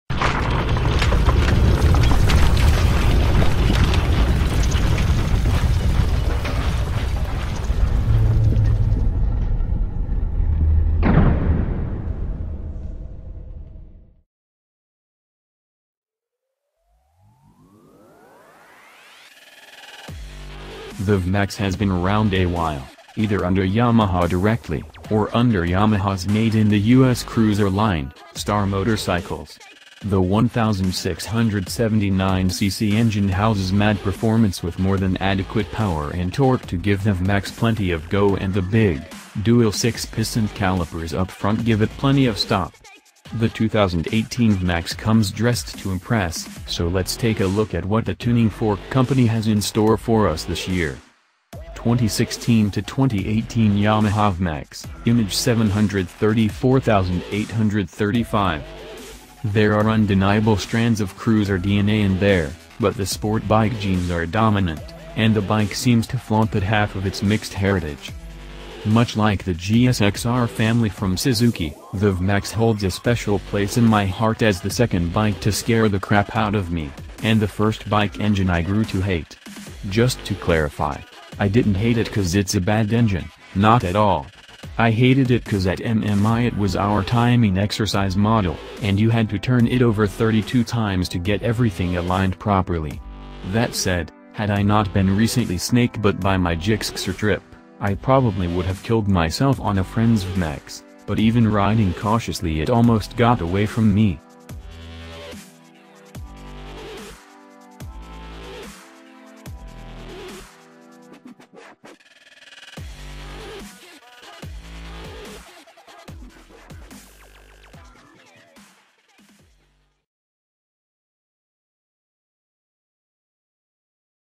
AWESOME !!!2017 Yamaha VMax Price & Spec
AWESOME !!!2017 Yamaha VMax Price & Spec - The VMAX has been around a while, either under Yamaha directly, or under Yamaha’s made-in-the-U.S. cruiser line, Star Motorcycles. The 1,679 cc engine houses mad performance with more than adequate power and torque to give the VMAX plenty of ’go’ and the big, dual six-piston calipers up front give it plenty of ’stop.’ The 2018 VMAX comes dressed to impress, so let’s take a look at what the Tuning-Fork company has in store for us this year.

██████ GET DAILY UPDATES! NOW! ██████